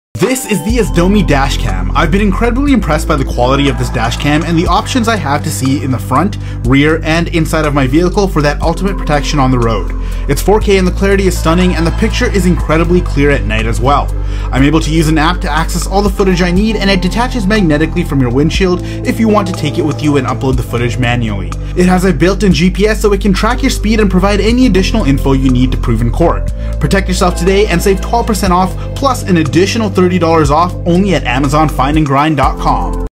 AZDOME M550 3 Channel Dash cams with WIFI & GPS Review
Protect yourself from fraudulent claims and accidents with the high-performance AZDOME M550 4K dash cam. Its G-sensor ensures that any sudden movements or impacts are automatically saved to prevent loss of important footage.

✅ 4K Resolution
✅ 3 Cameras - Front, Interior Cabin, Rear
✅ Detachable from mount
✅ Auto-record and Auto-shutter off

Video from @amazonfindandgrind